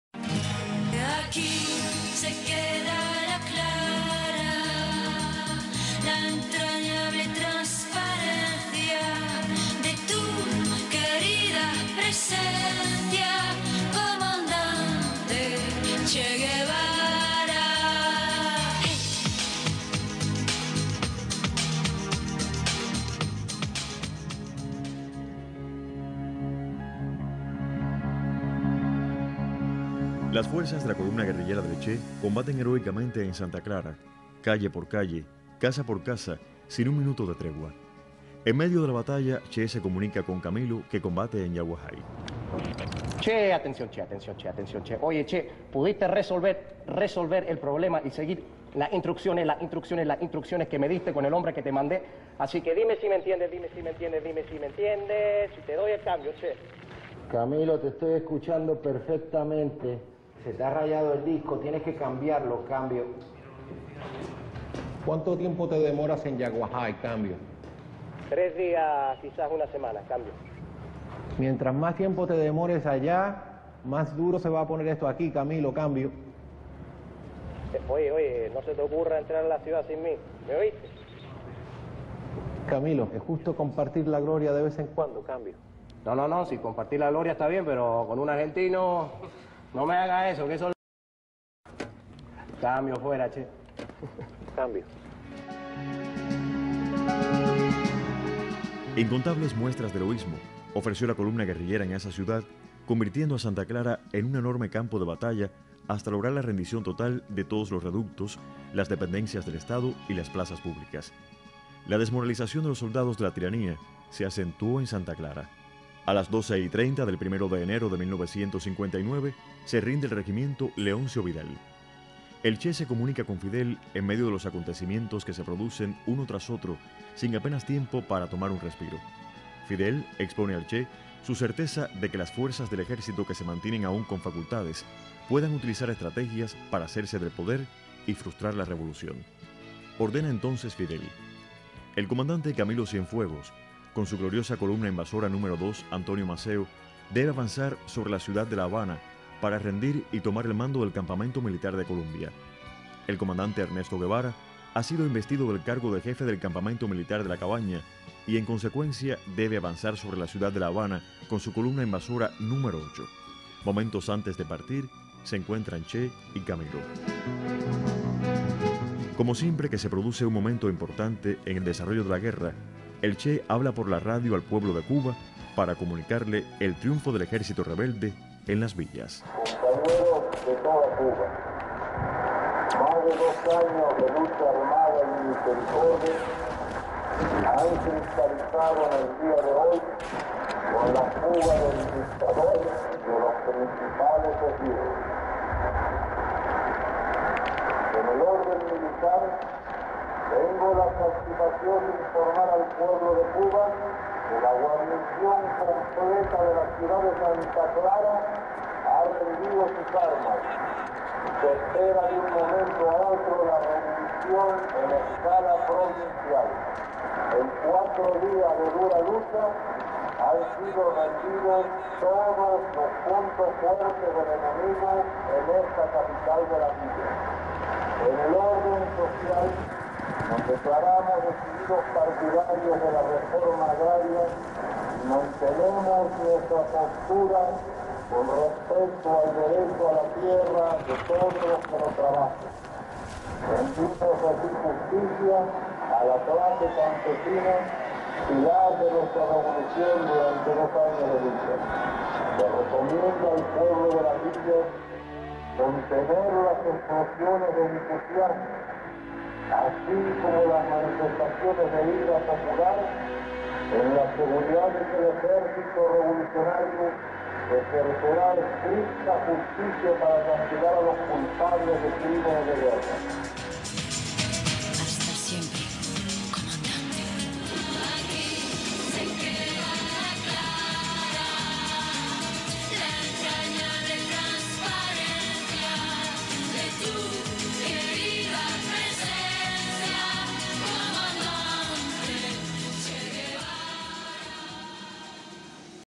El Che es investido como jefe militar de La Cabaña; y avanza sobre la Cuidad de La Habana.Capt48.
Miniprograma que tiene el objetivo de honrar la memoria del Comandante Ernesto CHE Guevara manteniendo su imagen viva en las nuevas generaciones.